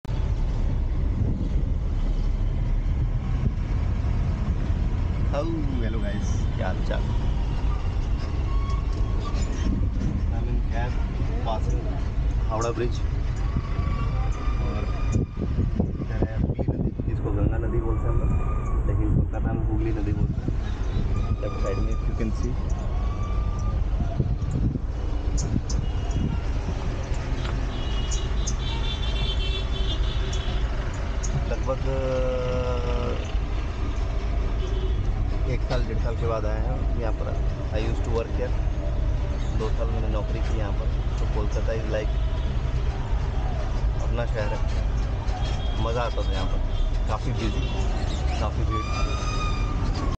crossing howrah bridge in taxi [360 video]
howarah bridge kolkata crossing in a can 360 virtual reality experience
behind the scene footate of my Indonesia travel